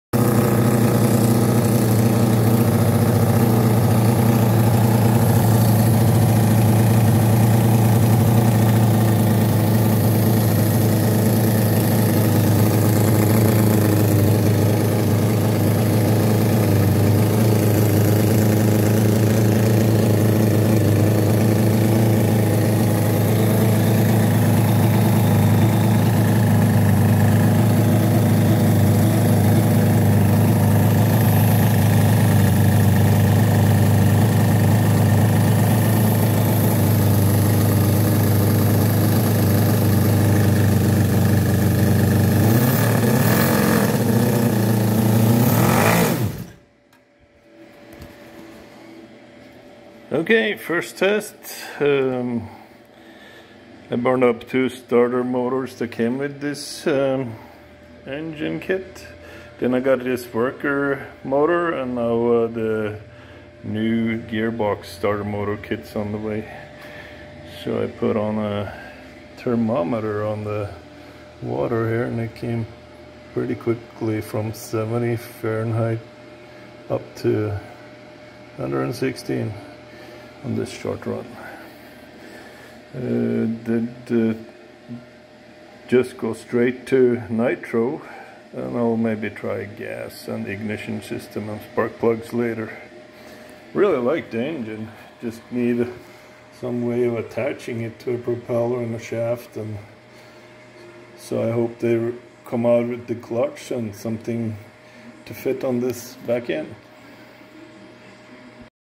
cison fl4-175 flathead engine first start, nitro.
First start of this engine from Cison and Stirlingkit. unsucessful attempt to start on gas with ignitionsysten and spark plugs , after burning 2 startmotors that came with kit.
 The Worker motor seems a better quality, and I went for nitro/ methanol and glow plugs, fired right up!
 Need to adjust the carb, and fix the oil leak from the pump, and run in the engine. Added a cheap thermometer, temp
climbed quickly from 70F to 116F in the few seconds the engine ran, will be intresting to se how high it will go, and if the fan makes a difference.
 Great little engine, should have painted it first like "steam212"